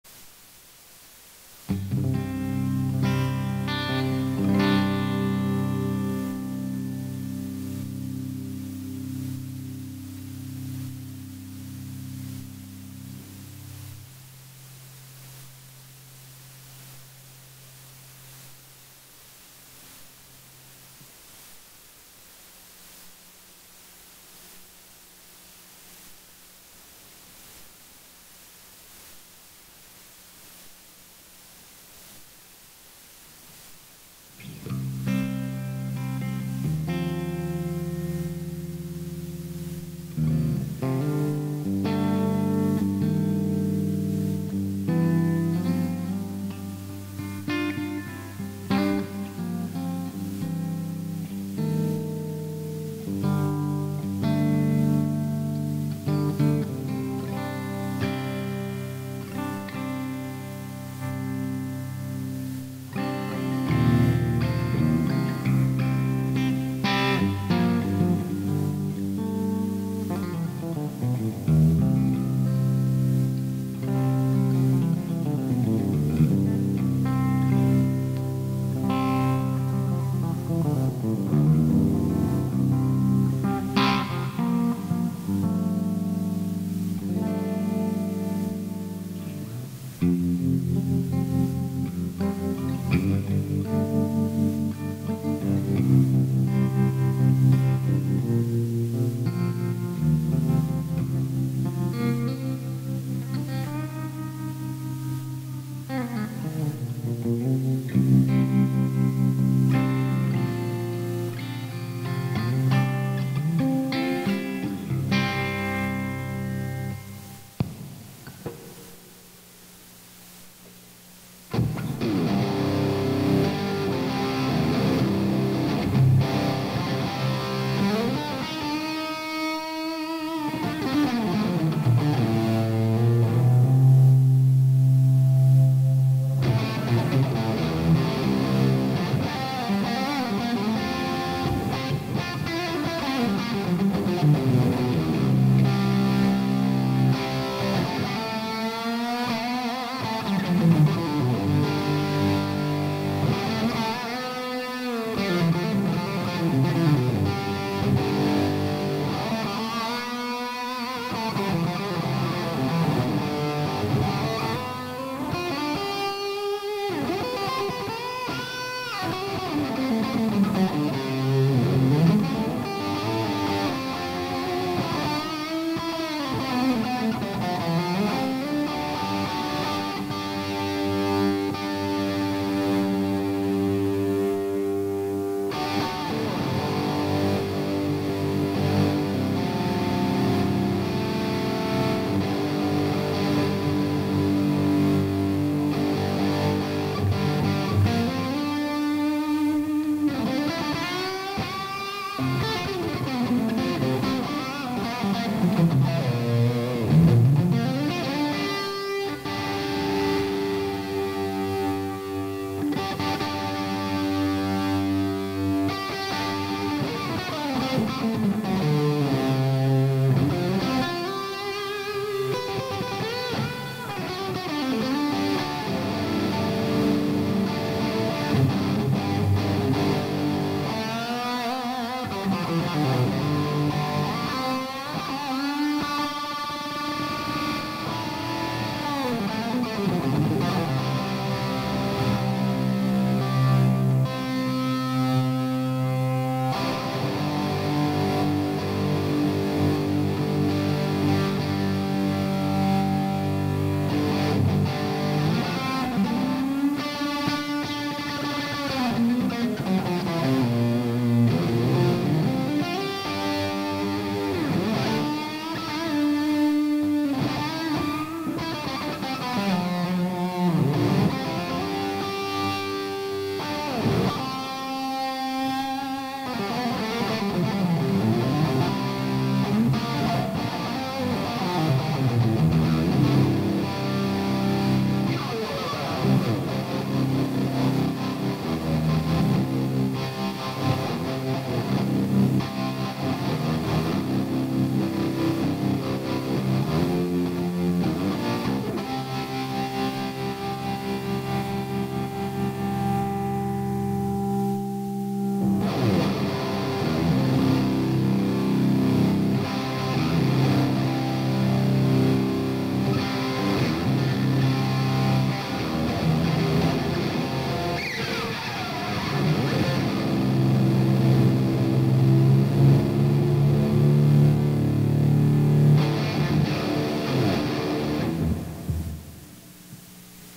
Fender GE Smith Tele/Esquire and Bogner Duende 1/2 Stack
Guitar has an RS Guitar Works Esquire kit.  Makes the "mud" setting musical and fully useable.  15 Watt Duende and Bogner cab has a pair of Scumback Greenies and a pair of Celestion Golds.